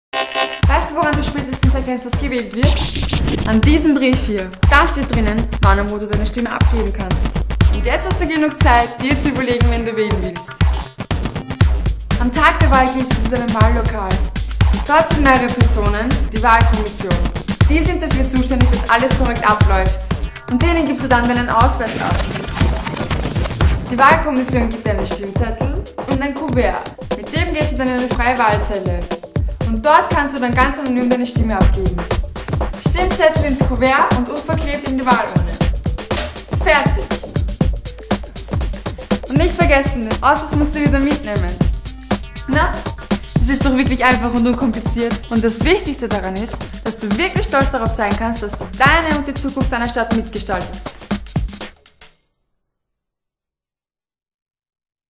Mit Austria's Next Topmodel Aylin Kösetürk zur Wiener Wahl 2010
Austria's Next Topmodel Aylin Kösetürk zeigt Dir, wie einfach Du bei der Wiener Gemeinderatswahl 2010 mitbestimmen kannst!  gibt's detaillierte Info zum Wählen..